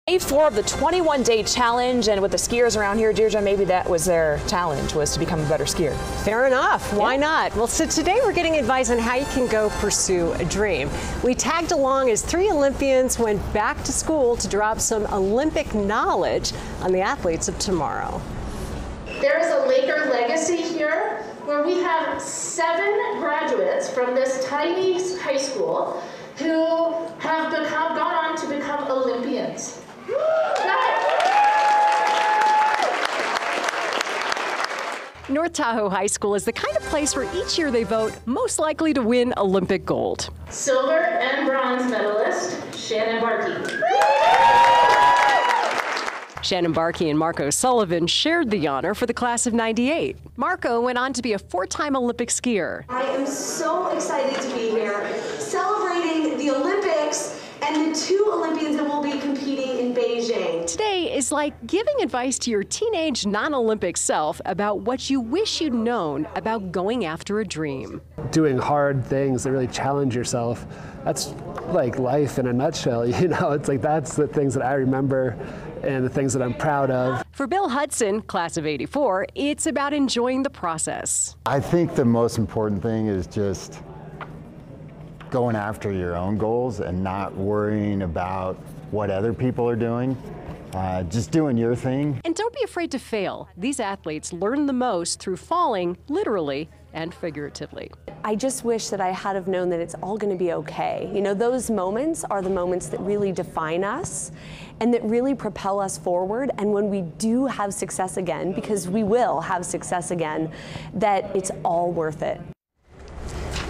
21-Day Challenge: Olympians share advice on how to go after a dream
It’s day four of the 21-Day Challenge and today we get advice on how to go after a dream. We tagged along as three Olympians went back to school to drop some Olympic knowledge on the athletes of tomorrow. North Tahoe High School is the kind of place where each year they vote for someone “most likely to win Olympic gold.”  Seven graduates from the small school have gone off to become Olympians.